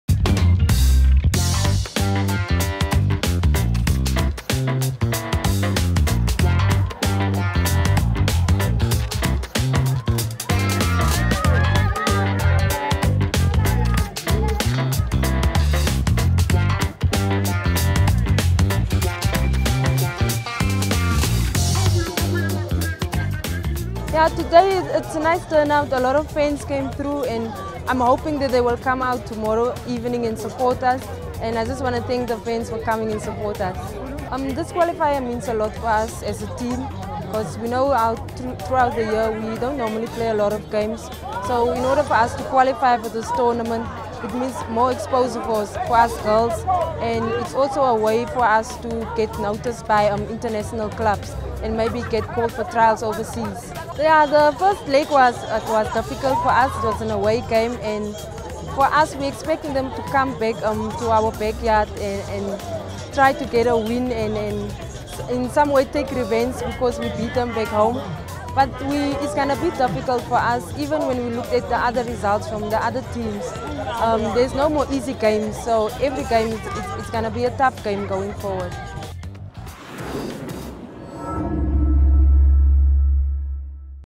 Sasol Banyana Banyana Tembisa Signing Session - 11 April 2016
Fans were out in full force to support Sasol Banyana Banyana ahead of their crucial qualifier against Botswana on the 12th of April 2016. Join the conversation by following us on Twitter